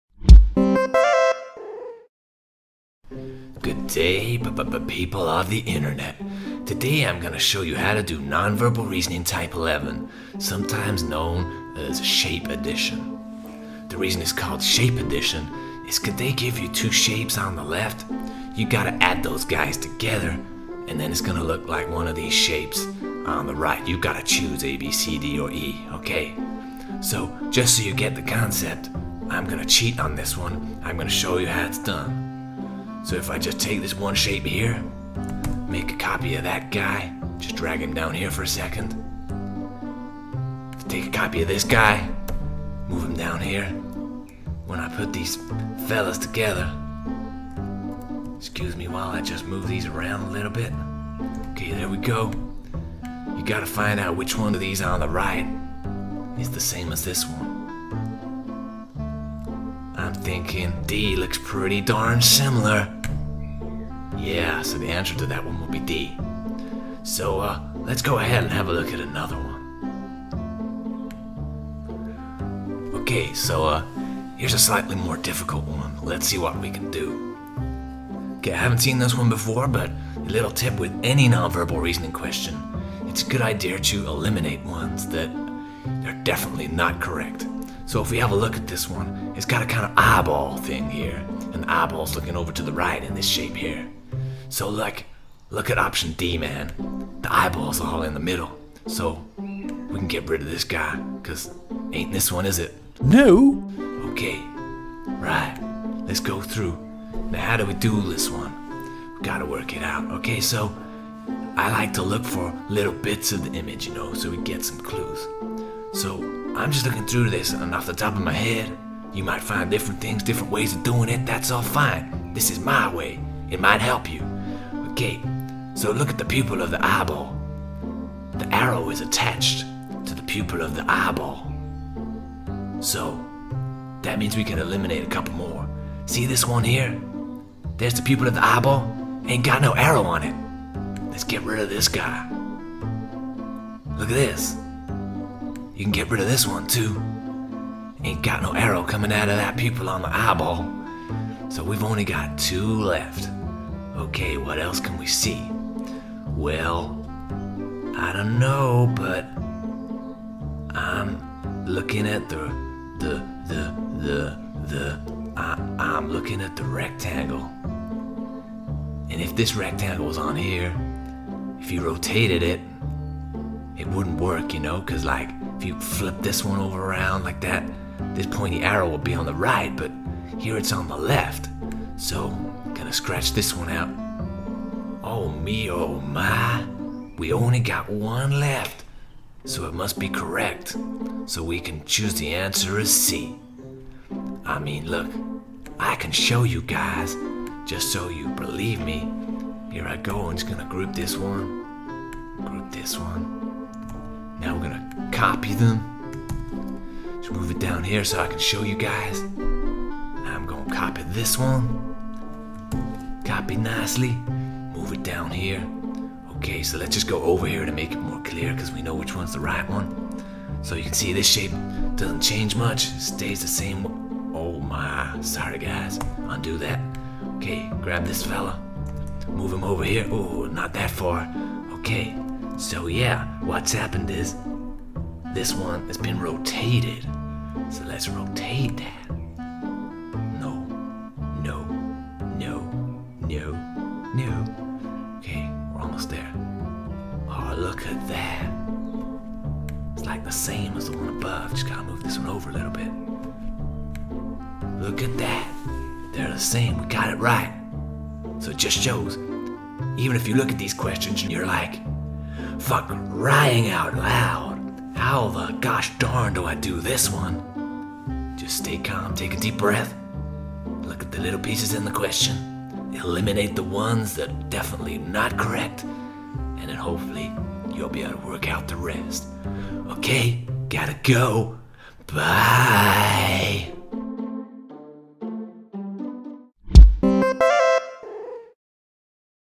NVR Type 11 - Shape Addition
Here's how to do NVR Type 11 - Shape Addition questions in 11 plus entrance exams.